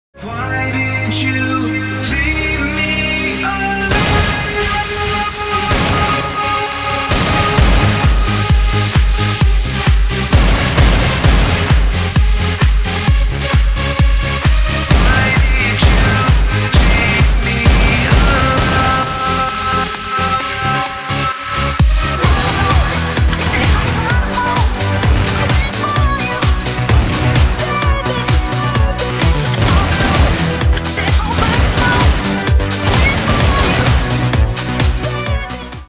Clayman by LuaN
It's some bugs(like the bloodeffectxD), and it was supposed to be longer but meh.. cba fixing it and re-render so here it is. The deathnotice change in the 2 part was just a test, going to use it in the future clips.